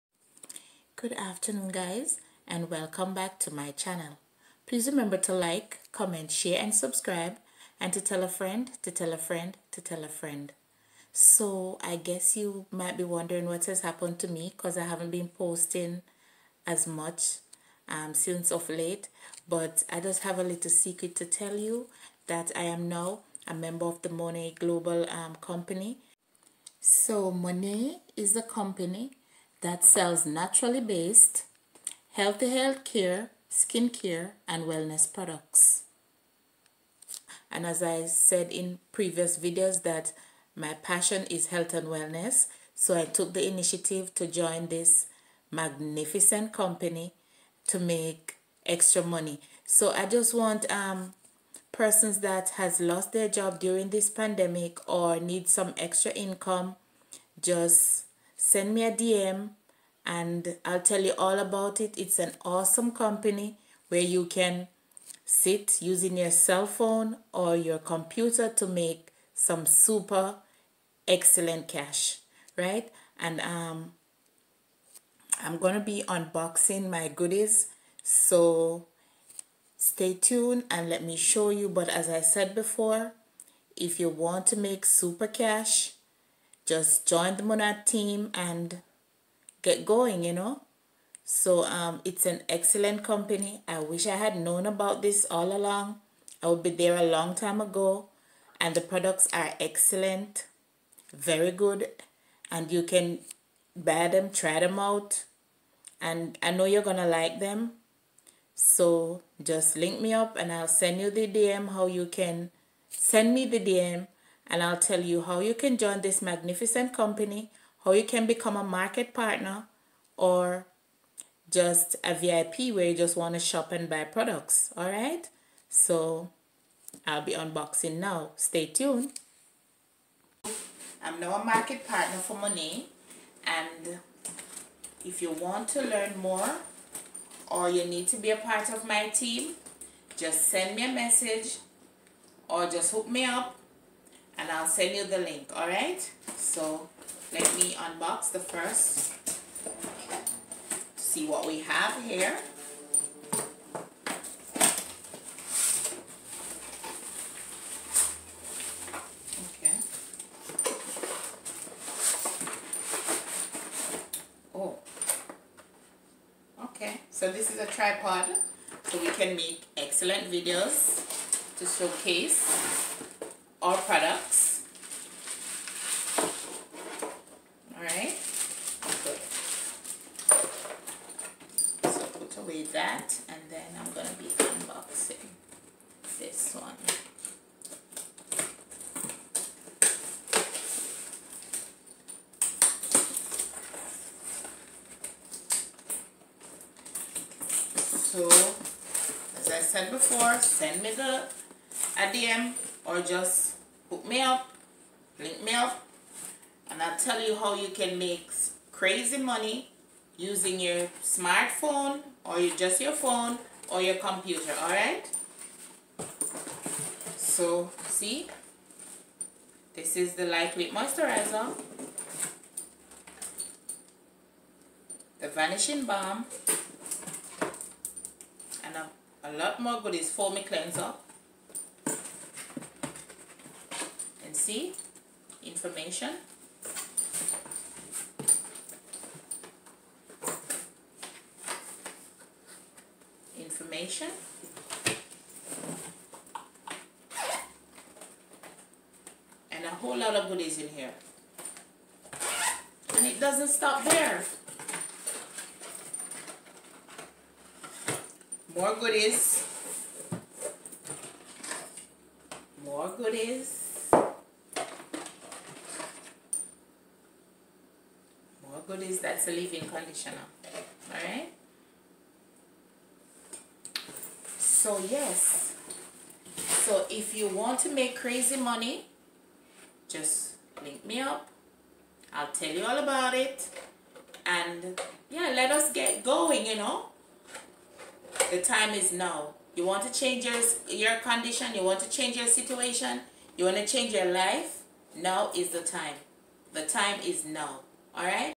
THE BEST BUSINESS DECISION I HAVE EVER MADE. MAKING MONEY USING MY COMPUTER!!
Working at this company has being the best decision I have ever made. I am able to make money by using my phone or my computer. This job came the right time during this pandemic.